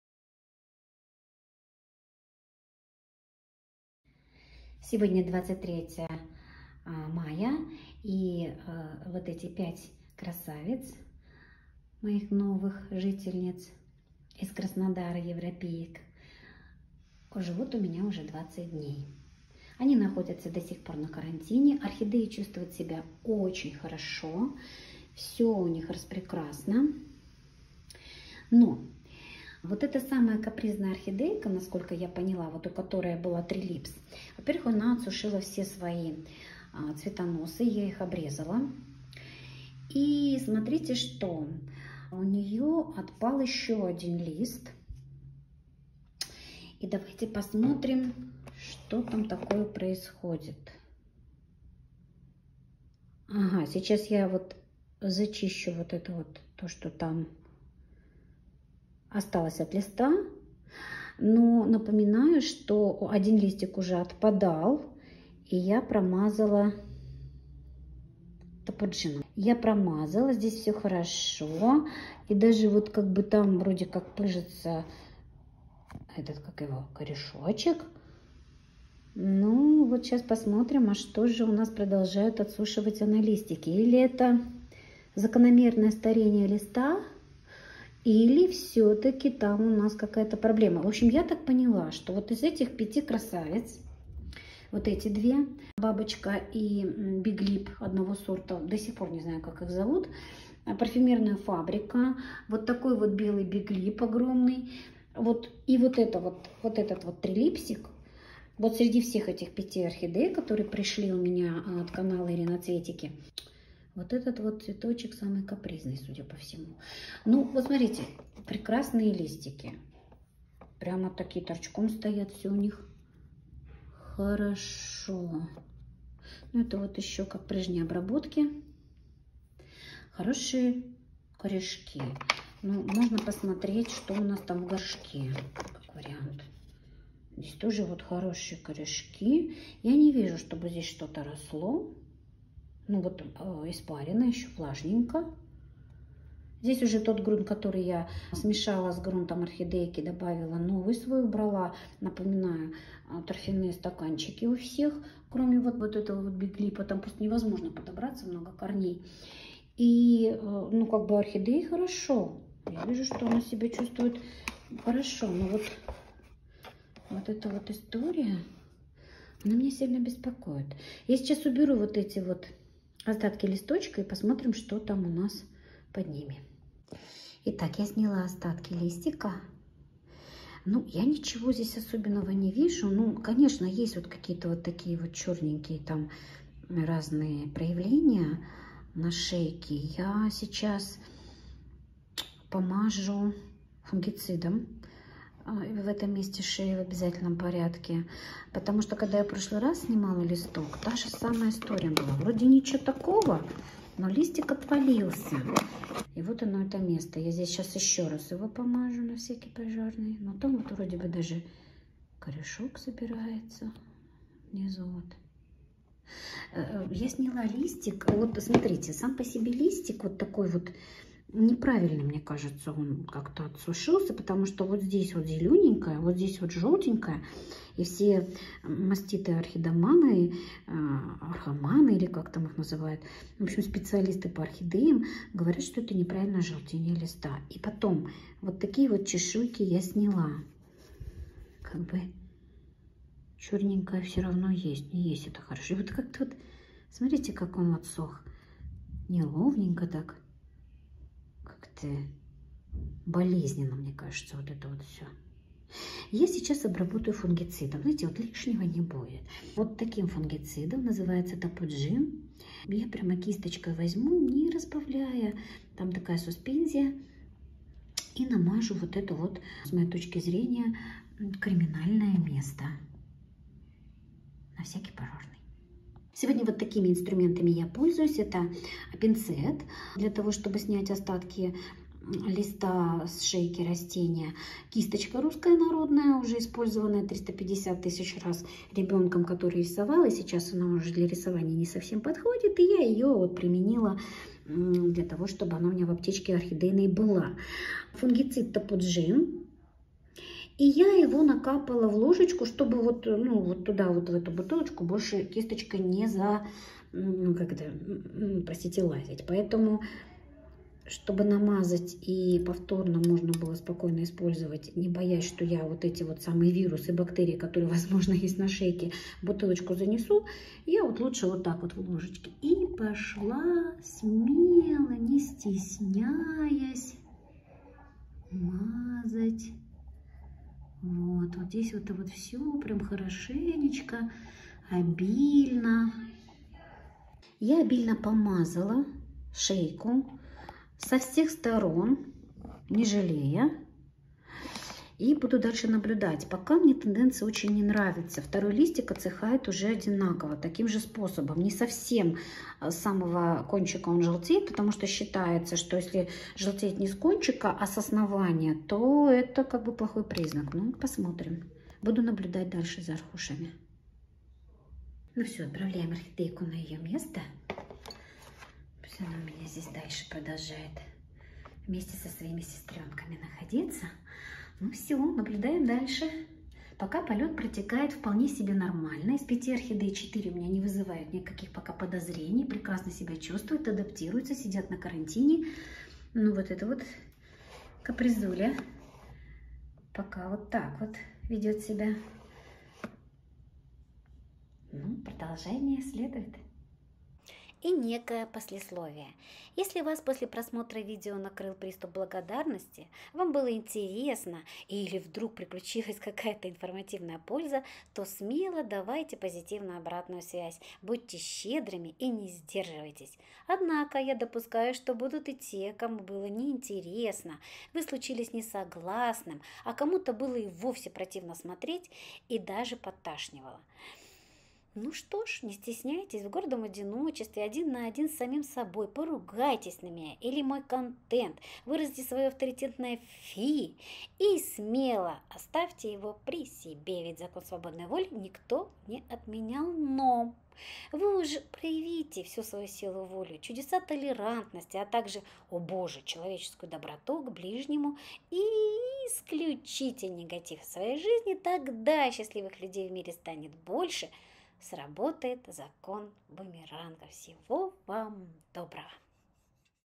#4 День 20. Европейские орхидеи из Краснодара от Ирины Цветики. Капризы, орхидея теряет листики
У меня получается некий видео дневник)
5 европейских орхидей из Краснодара от канала Ирина Цветики продолжают быть на адаптации.
В этом видео:
🌸 4 орхидеи чувствуют себя отлично, а вот одна орхидея пелорик трилипс скинула три листика.
🌸  Скорая помощь по спасению орхидеи.
🌸. Пользуюсь фунгицидом Топуджин для блокировки потенциального заболевания одной орхидеи.
🌸. Восстановление орхидей после пересылки с юга на Урал продолжается (7 дней орхидеи ехали ко мне в тёмной коробке).